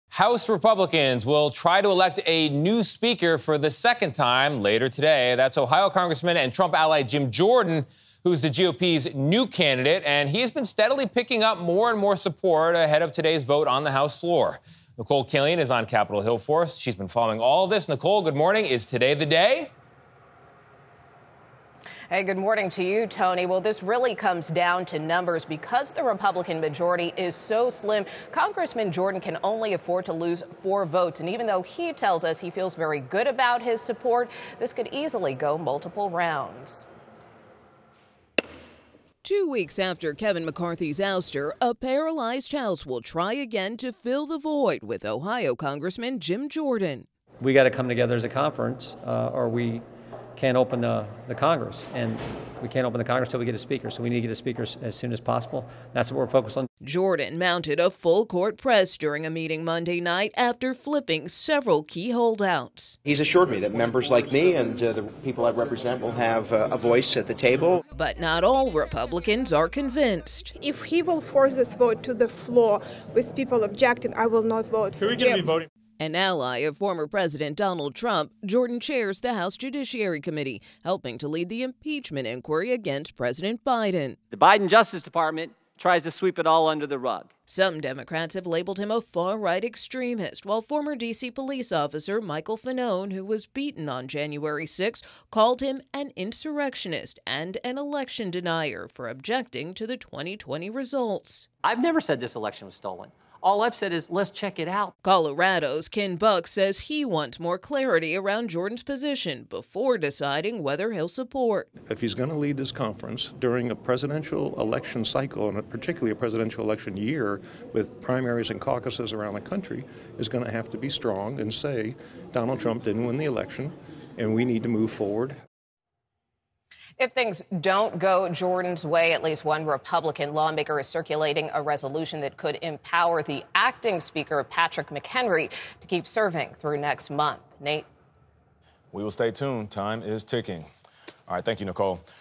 House to vote for new speaker as Rep. Jim Jordan tries to win enough support
Republican Rep. Jim Jordan will take his bid to become speaker of the House to the floor on Tuesday. Whether or not he has enough votes to take the gavel remains to be seen. CBS News' Nikole Killion reports from Capitol Hill.